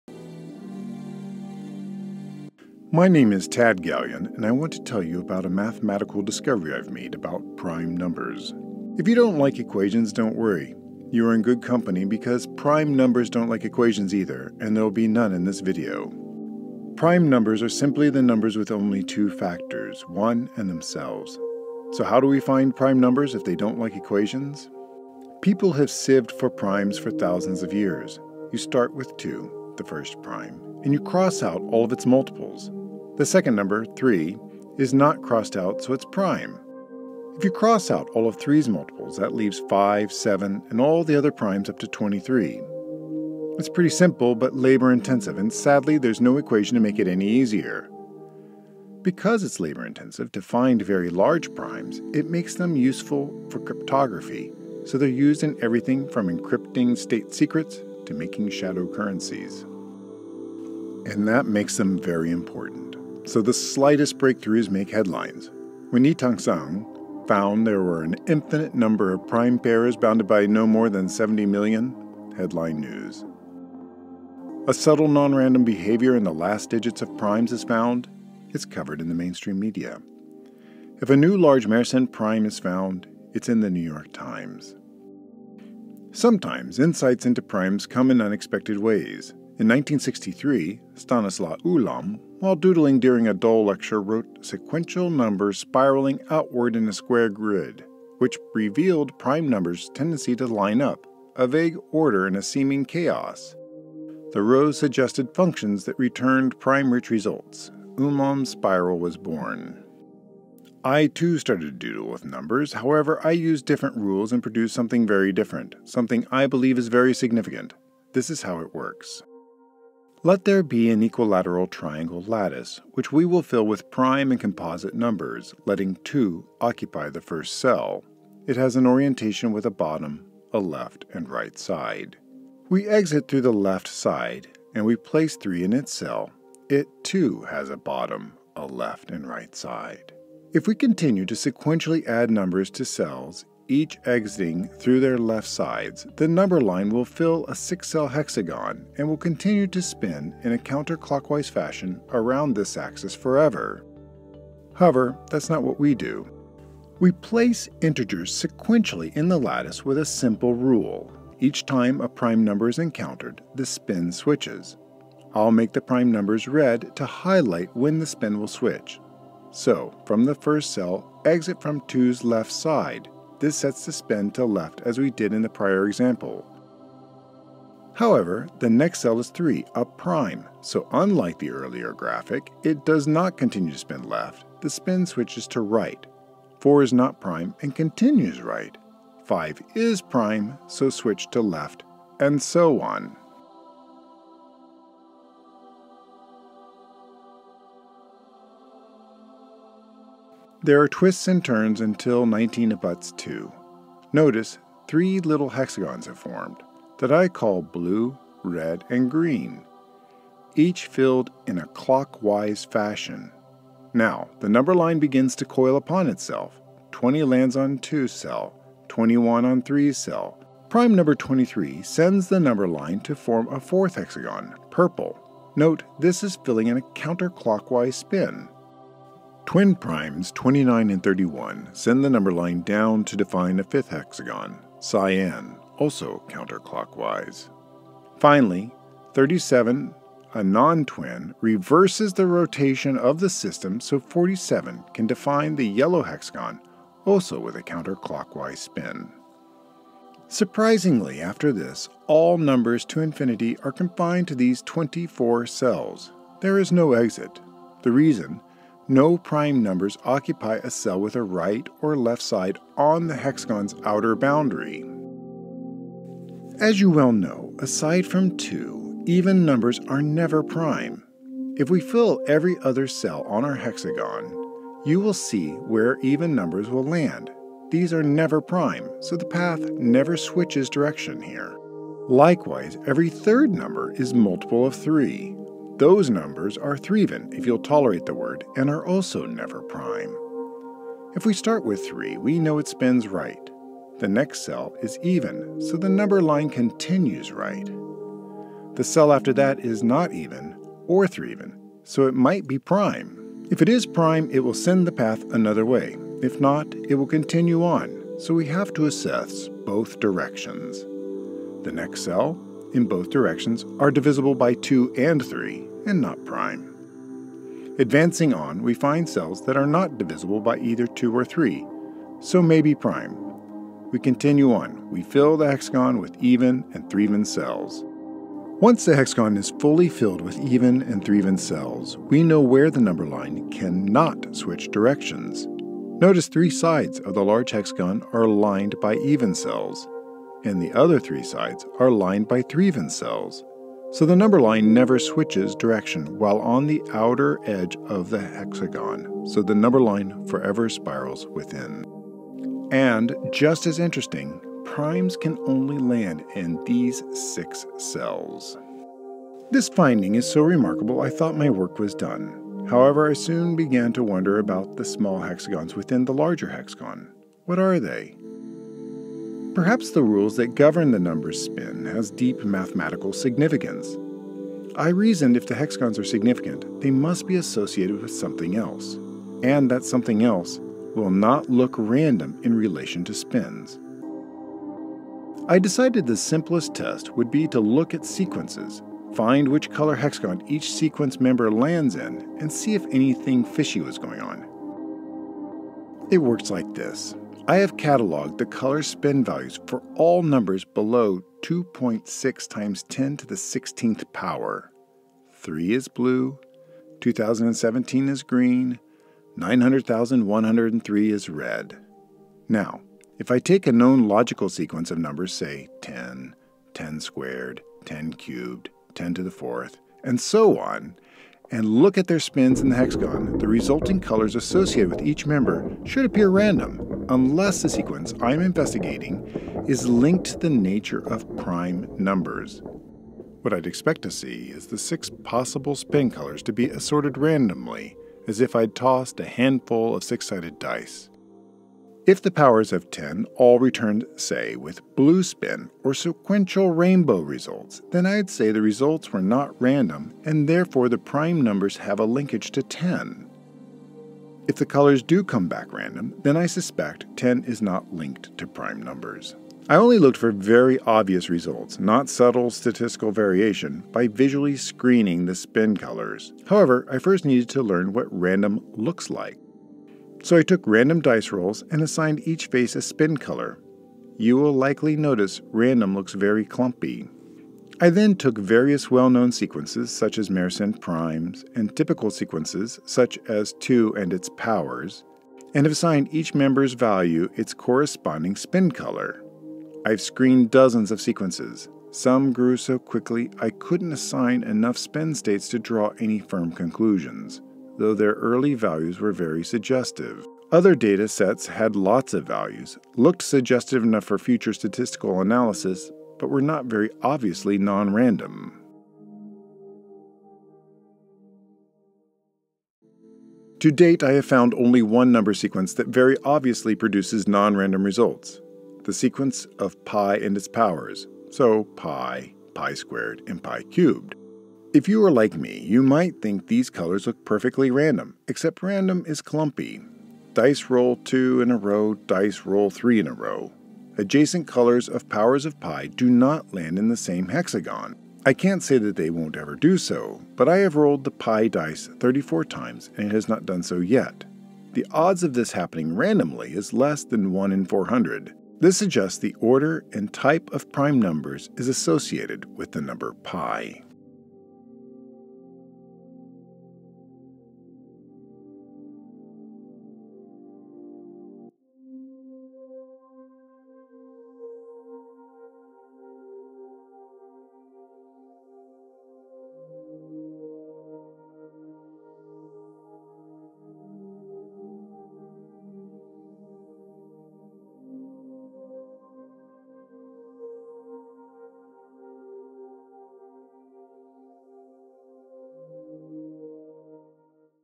Prime Hexagon - Introduction
This video is about the Prime Hexagon.  For more information about my work go to Hexspin.com.  I welcome thoughtful, friendly critiques.
Clarification and corrections -
1.  When scripting the video I inadvertently referred to the n value for the prime sequence, rather than the power - which are one off of each other.  I've not yet rolled the 'pi dice' 34 times - I've only done 33.  34th roll results should be out soon - early April.
2.  Truncated powers of pi - these are the powers of pi, with the fraction clipped off AFTER the power is taken See "22 digit" precision data on my website.
3. My words 'this suggests the order and type of prime numbers is associated with the number pi' mean the data suggests a connection. This is not a conclusion, it is a suggestion.  I'm working on expanded data set to strengthen that suggestion.  I'm not sure I can 'prove' a connection - but I believe I can build a strong, interesting case there is one - or show the first of the pi data is just a fluke.  More to come.